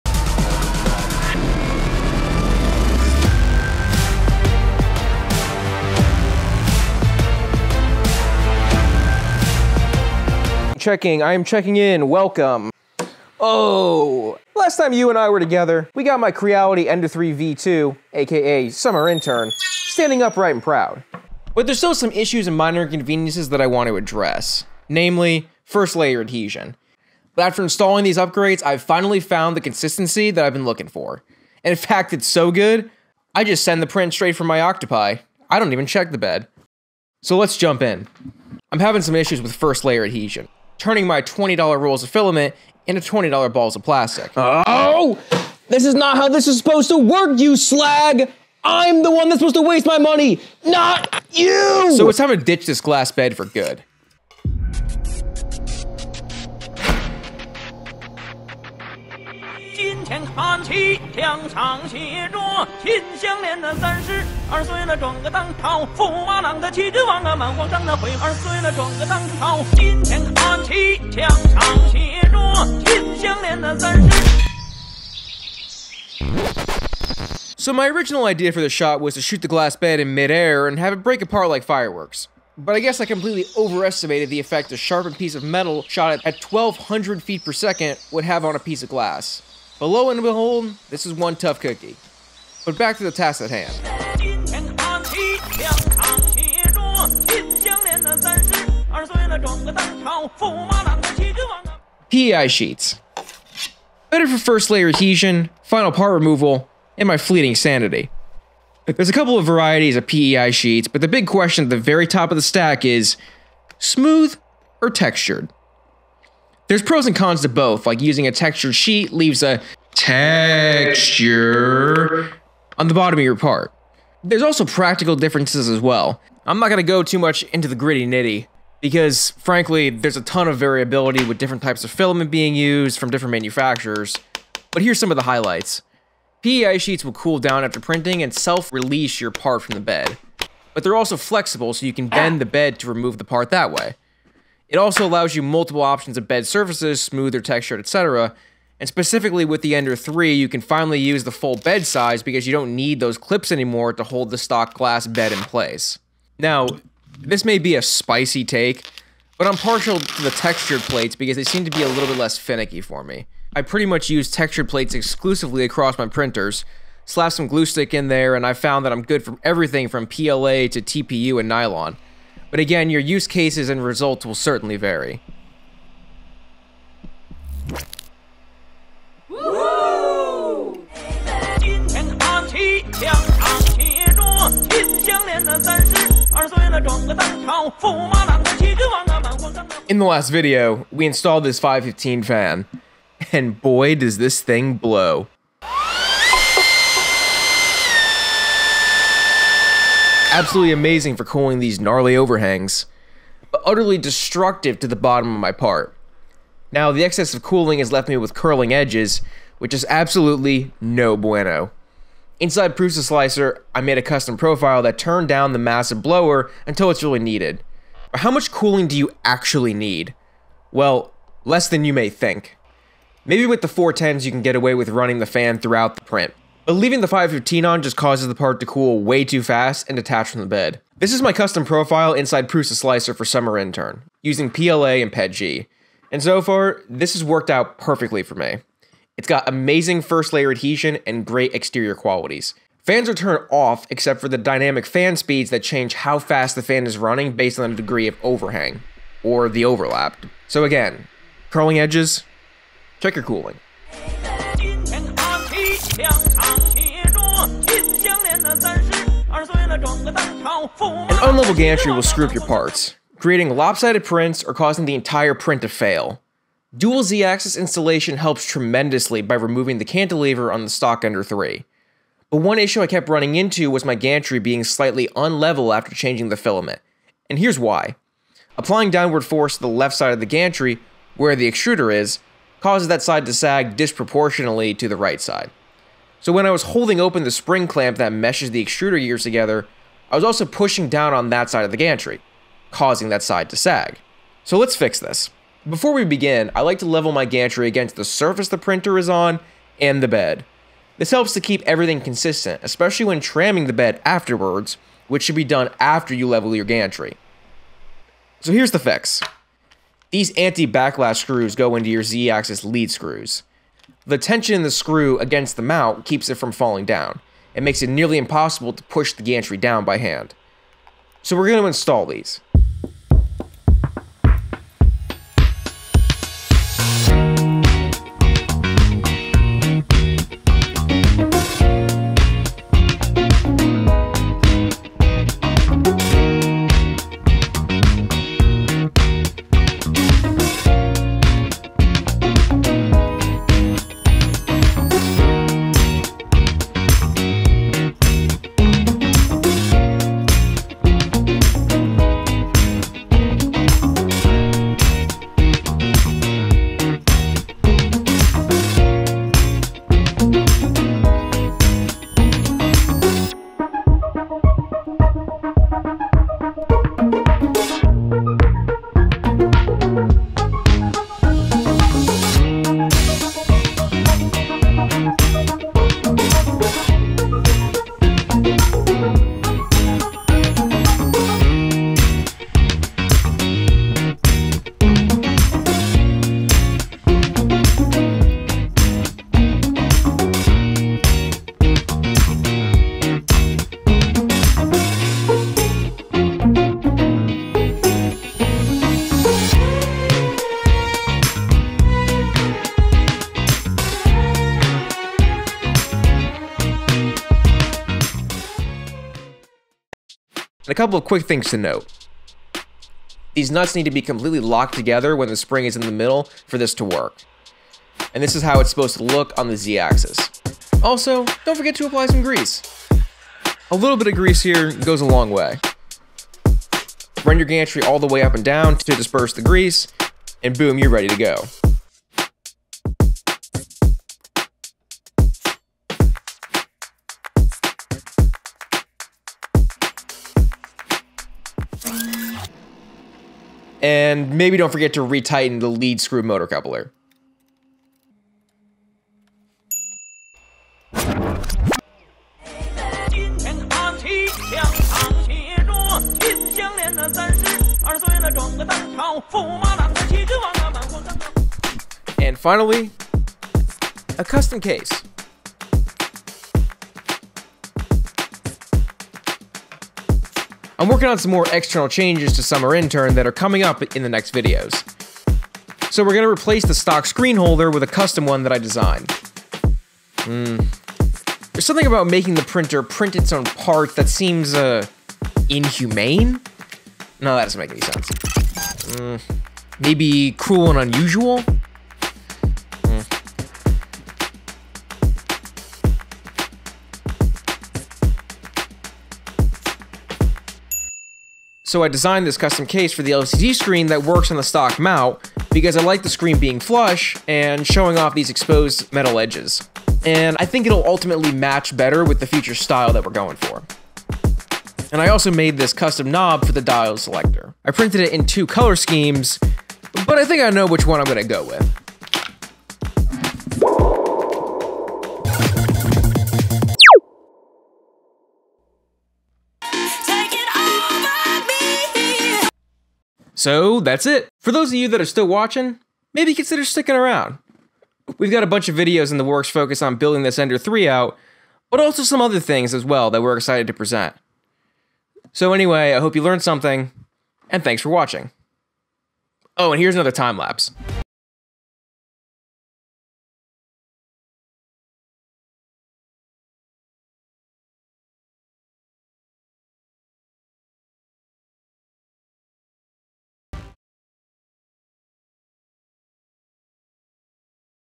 CREALITY Ender 3 V2 Build Series | First Layer Adhesion
Oh Boy, WE'RE BACK!!

This time around, we're taking a look at some upgrades and changes to give better first layer adhesion. Let's make those parts stick!

-Maxy

Like what I'm creating? Follow me on...